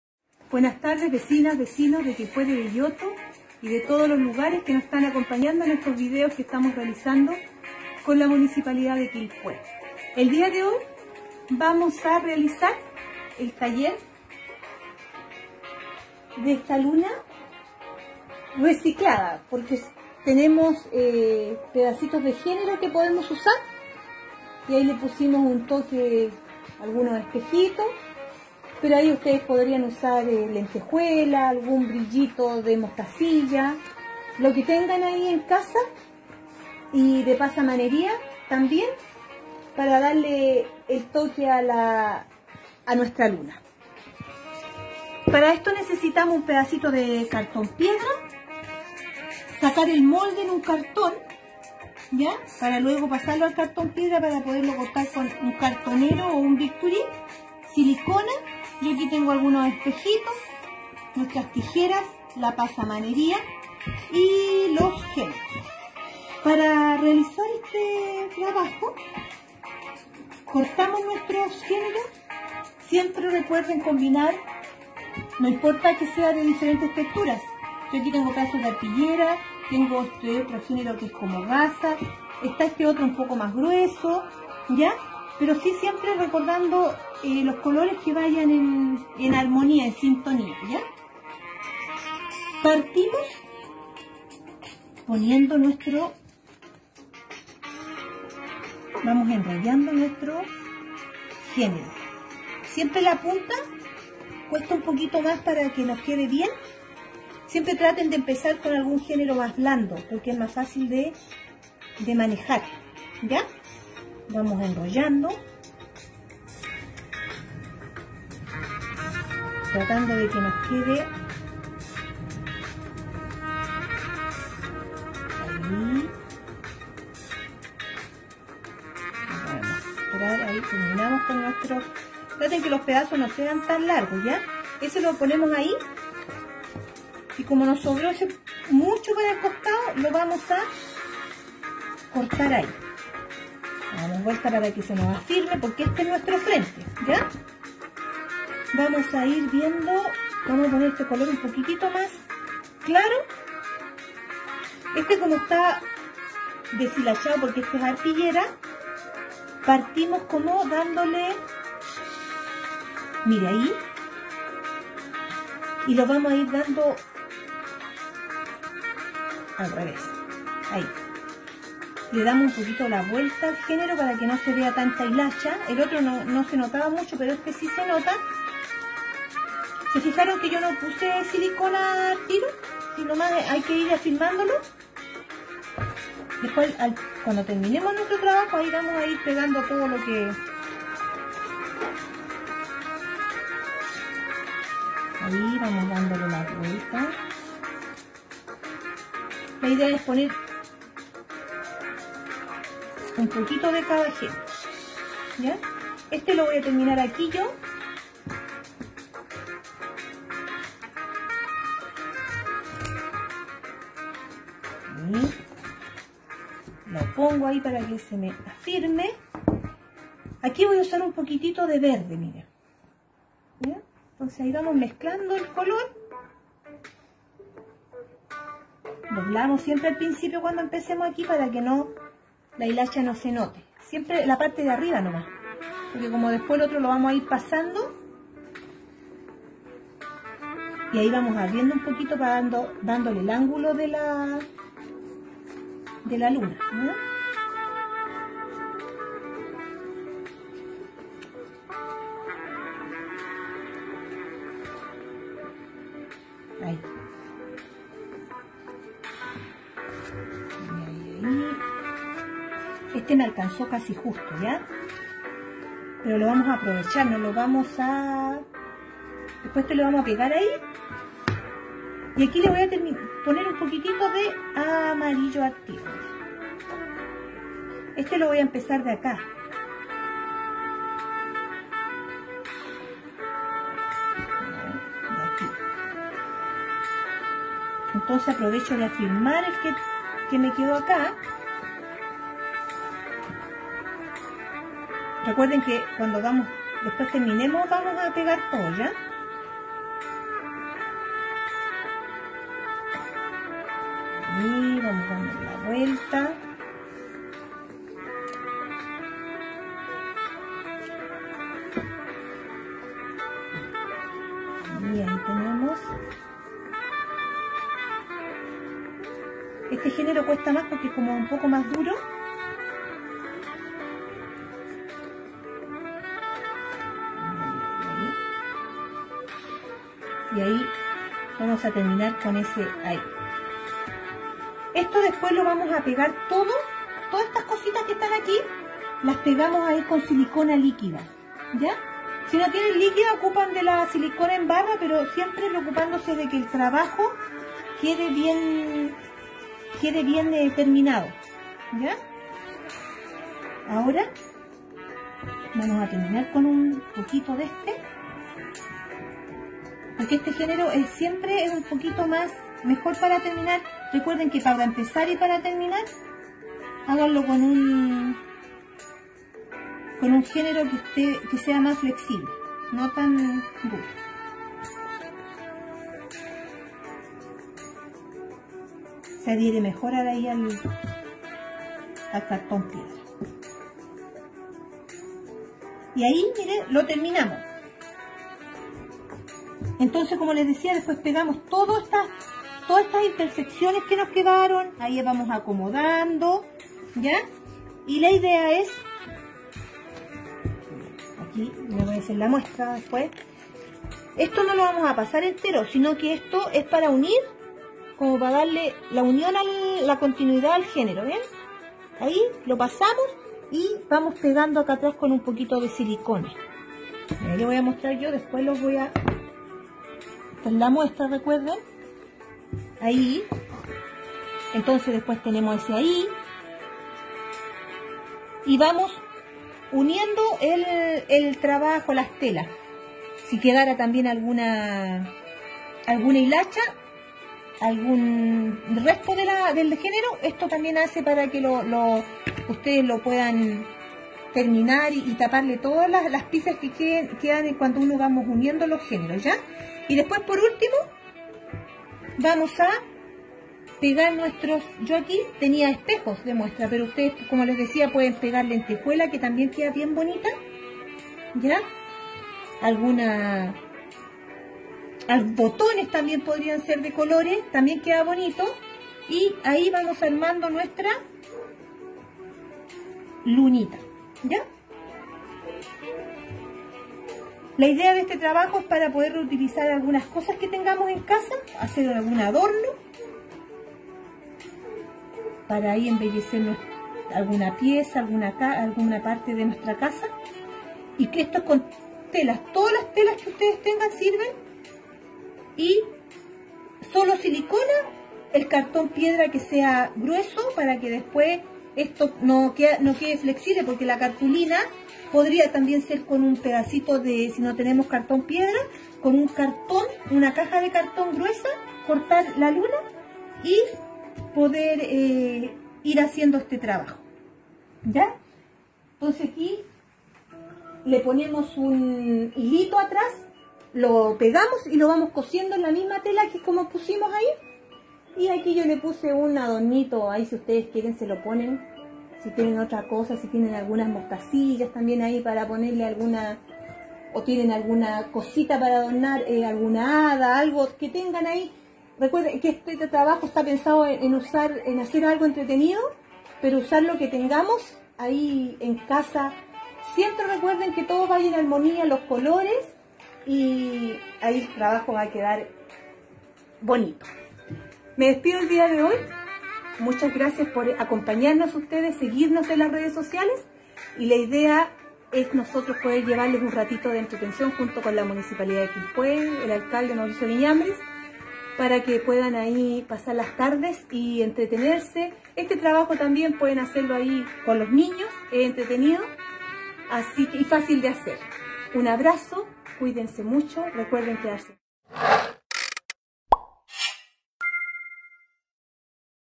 Taller: Luna adorno - Viernes 28 de agosto 2020
Hola amigos y amigas de Quilpué y El Belloto!

Esperando que estén todas y todos bien en sus casas, comenzamos otra jornada de Talleres de Manualidades, organizados por la Municipalidad de Quilpué, gracias a la iniciativa del alcalde Mauricio Viñambres.
Para hoy viernes, les dejamos con el video tutorial de nuestra monitora Yolanda Caro, quien nos enseñará a diseñar una luna de adorno para nuestro hogar. ¡Que lo disfruten!
Les compartimos el paso a paso de esta técnica:
1.Cortar molde en cartón piedra o caja
2.Cortar telas en tiras e ir pegando en el cartón
3.Colocar pasamaneria para que uniendo la telas
4.Decorar con brillos , lentejuelas, espejos, botones
¡Que tengan un excelente fin de semana! 😊